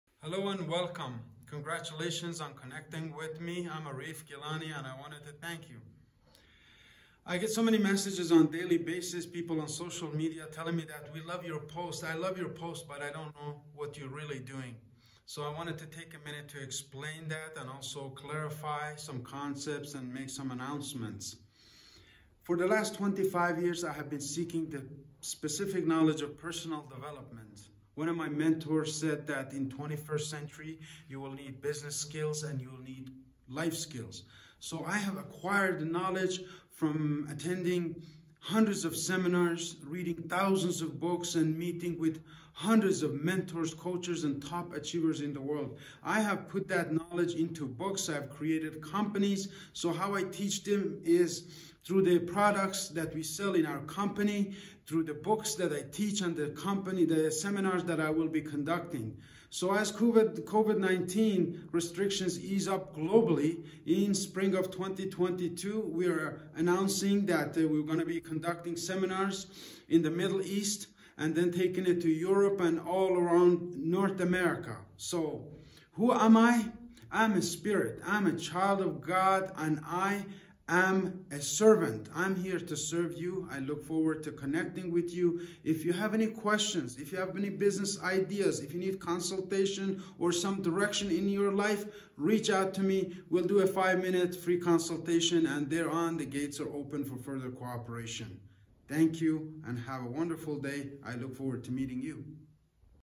Gilany Leadership Academy The Ministry of Miracles 2022
Relaunch of our companies with the opportunity for you be participate in creating miracles in your own life.

I look forward to sharing some life changing concepts with you and hearing your business idea, venture and or products ideas that will change the world for the better.
Arif Gilany,
The Ministry of Miracles
Gilany Leadership Academy